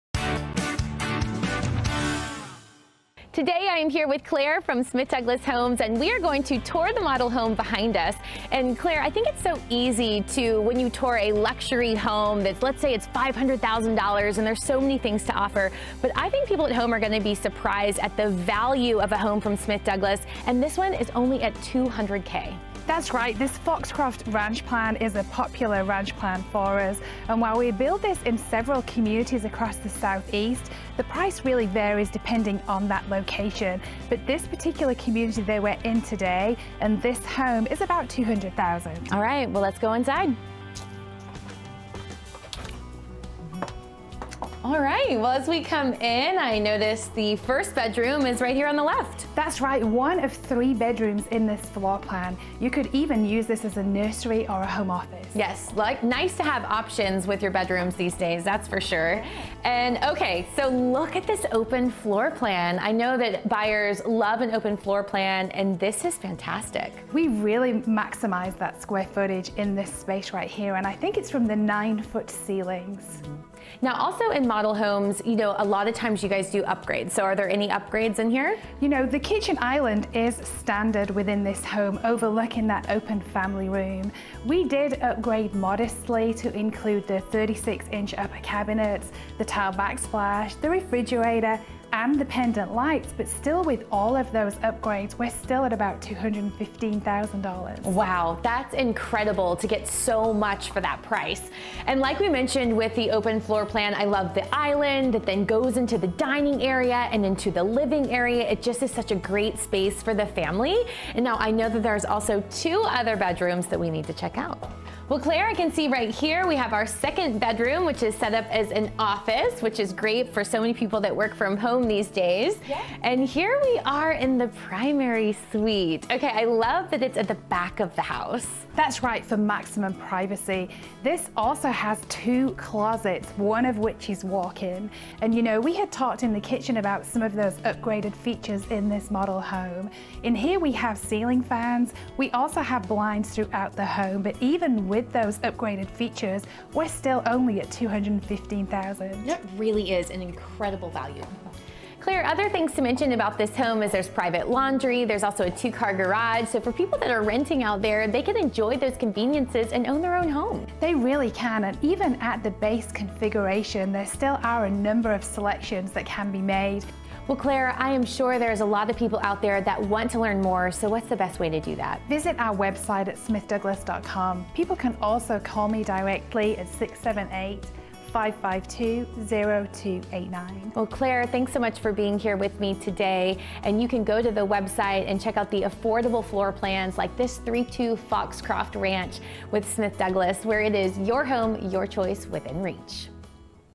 Discover What You Can Get For Under $300k with Smith Douglas Homes
Discover how affordable our Ranch Style, Foxcroft model can be. This 1,202 square foot Foxcroft ranch is offered in several communities across the Southeast and, while the base price varies depending on location, this particular home at Collington in Dalton, GA is featured starting below $200k!

Many of our home buyers have never owned a home before, let alone a brand new one.  While it’s easy to show off a $500k luxury home, today we’re going to showcase some of the great features of this $200k home that you don’t pay extra for and demonstrate just how much home value you get for your money with Smith Douglas Homes.

The Foxcroft starts with a lovely, open floor plan that buyers demand these days.  The design maximizes use of space and with its 9’ ceilings, if feels like a larger home. Standard in this home is this spacious kitchen island overlooking the living & dining areas, with seating for 4. There are 3 bedrooms and two full bathrooms, with separation between the primary suite, tucked away for greater privacy at the back of the home, and a secondary bedroom up front that’s perfect for children or guests. The primary bedroom features its own private en suite bath and two spacious closets. The third bedroom makes for a great home office or nursery. Plus, an attached, 2-car garage with direct access into the home is a welcome feature on foul-weather days.

Imagine if you’re currently renting a place without a garage or private laundry facility and discovering you can get all this standard in the affordable Foxcroft plan! Of course, as with all our home designs, buyers of the Foxcroft can get this plan in its base configuration and still express their personal sense of style by selecting from a number of no-extra charge finishes, such as flooring, cabinetry and exterior color. Find the Foxcroft or plans like it at a community near you.

If you're in the Dalton, GA area, you'll love the charm of Collington, with quick access to downtown Dalton, numerous walking trails and parks, as well as quaint shops and eateries. Visit us at the Foxcroft Model today to see if Collington is the right place for you to call home. At Smith Douglas Homes, it's Your Home. Your Choice. Within Reach!